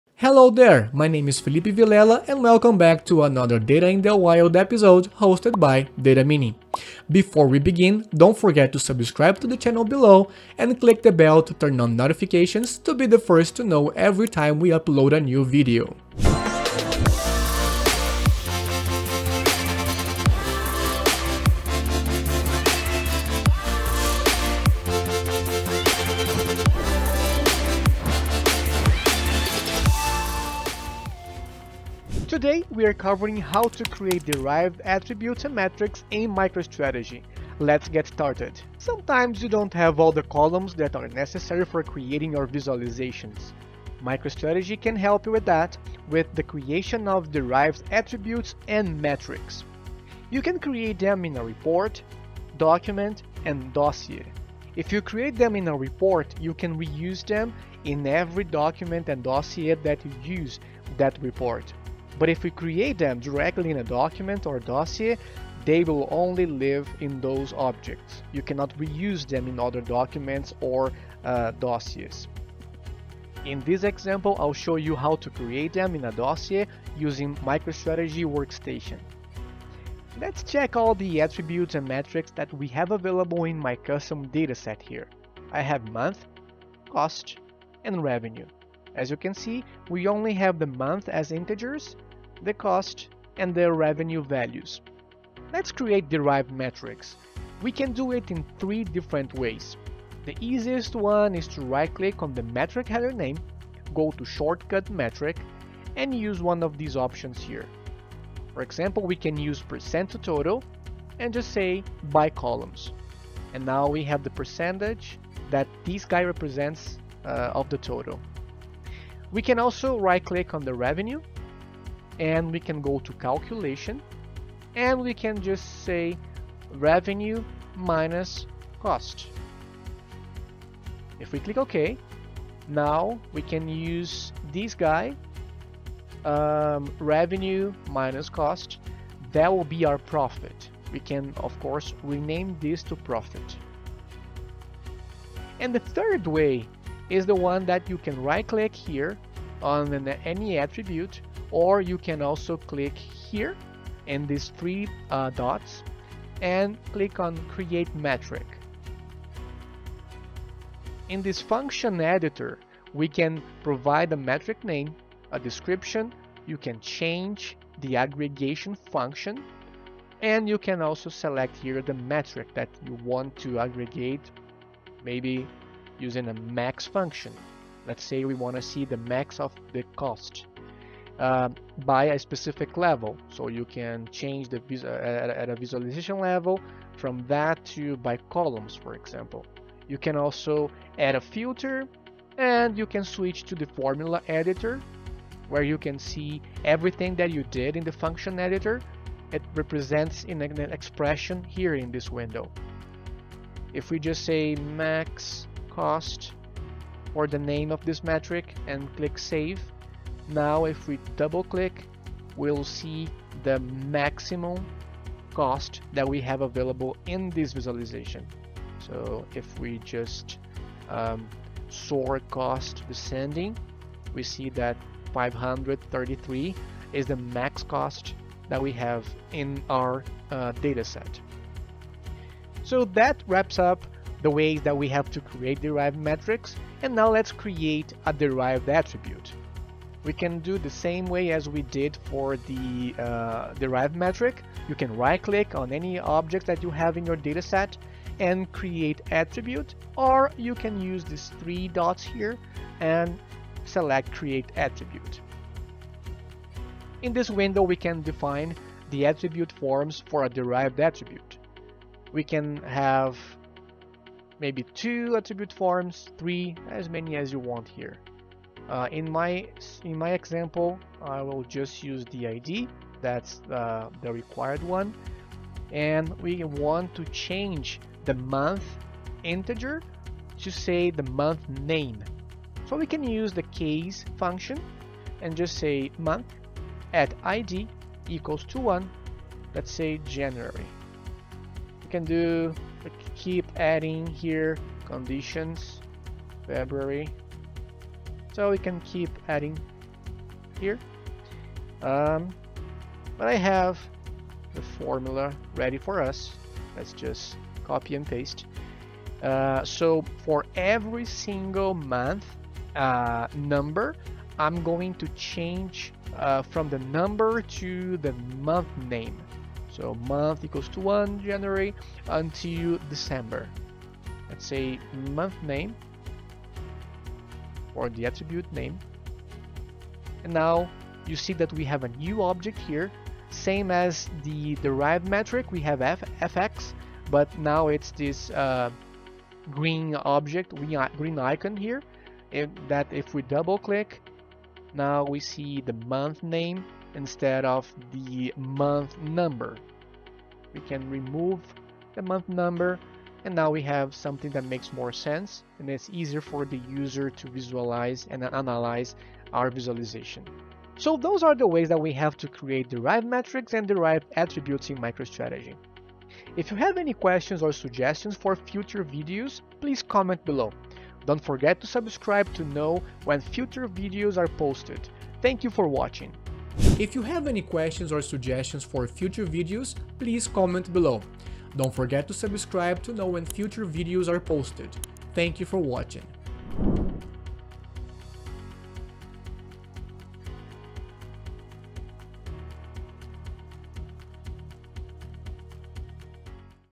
How To Create Derived Metrics and Attributes in MicroStrategy
In this Data in the Wild episode, you'll learn how to create derived metrics and attributes in MicroStrategy.